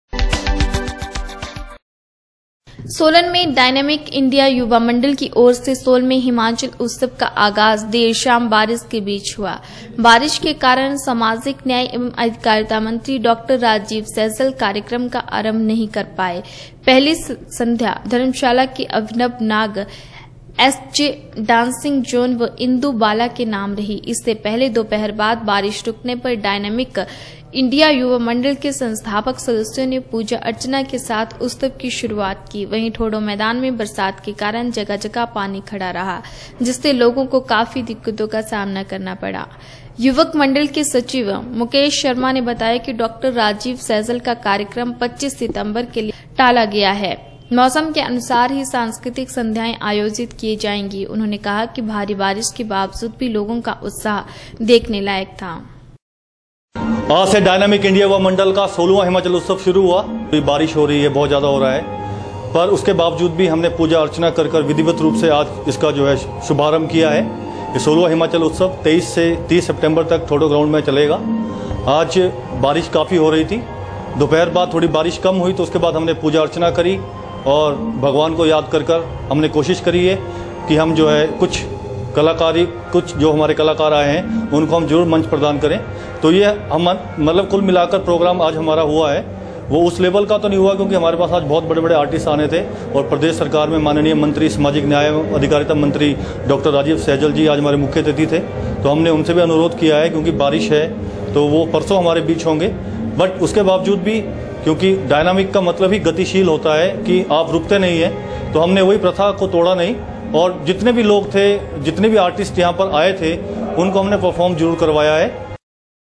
इंडिया युवा मंडल की ओर से 16वें हिमाचल उत्सव का आगाज देर शाम बारिश के बीच हुआ
सोलन में डायनामिक इंडिया युवा मंडल की ओर से 16वें हिमाचल उत्सव का आगाज देर  शाम बारिश के बीच हुआ। बारिश के कारण सामाजिक न्याय एवं अधिकारिता मंत्री डॉ. राजीव सैजल कार्यक्रम का आरंभ नहीं कर पाए। पहली संध्या धर्मशाला के अभिनव नाग, एसजे डांसिंग जोन व इंदू बाला के नाम रही।इससे पहले दोपहर बाद बारिश रुकने पर डायनामिक इंडिया युवा मंडल के संस्थापक सदस्यों  ने पूजा अर्चना के साथ उत्सवकी शुरुवात की।